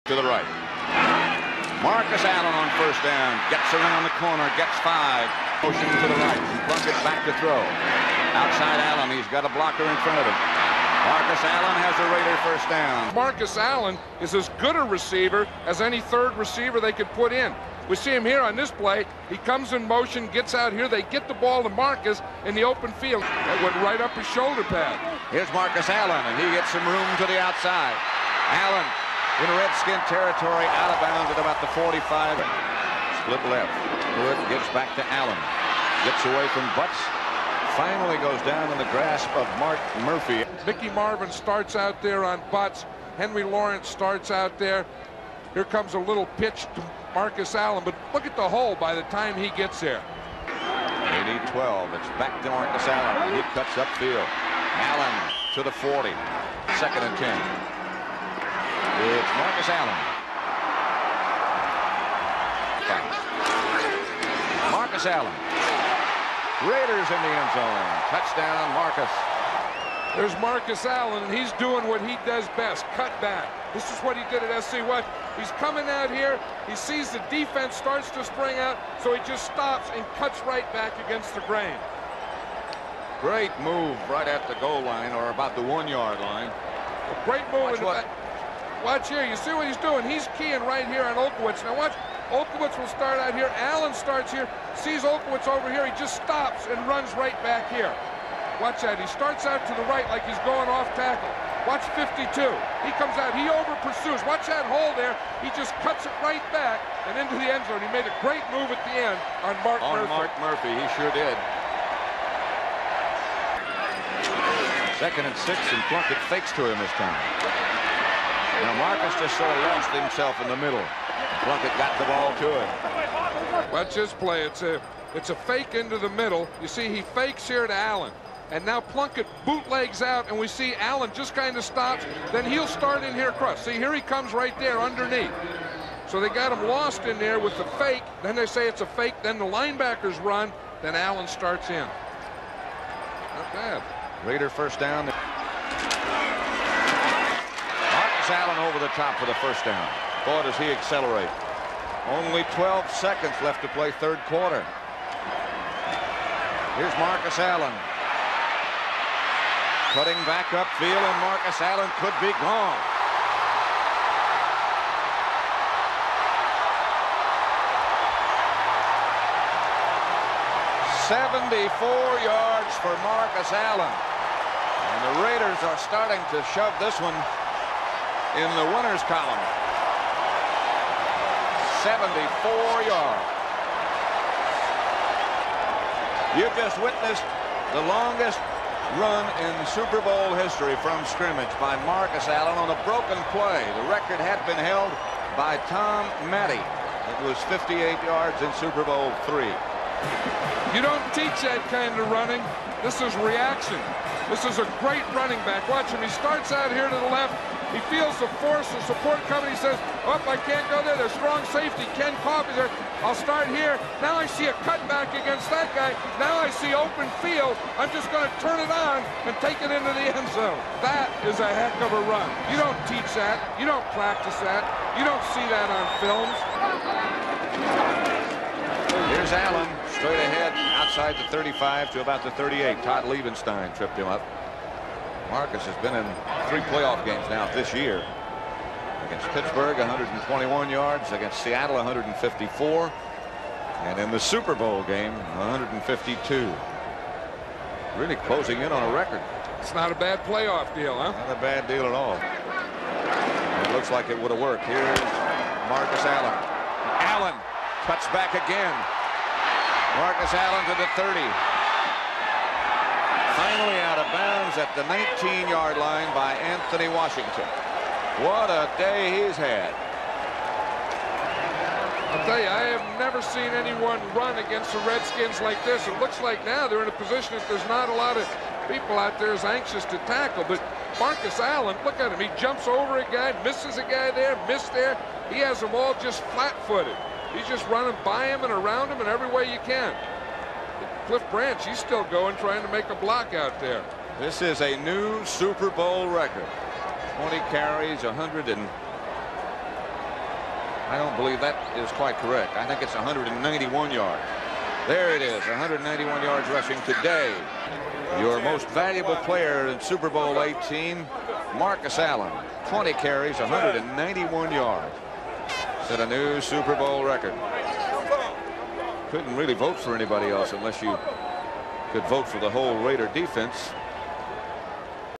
Highlights: Marcus Allen Goes off in Super Bowl XVIII, Wins MVP | Las Vegas Raiders
RB Marcus Allen made Super Bowl history with his 191-yard rushing day and iconic 74-yard touchdown run against the Washington Football Team in Super Bowl XVIII.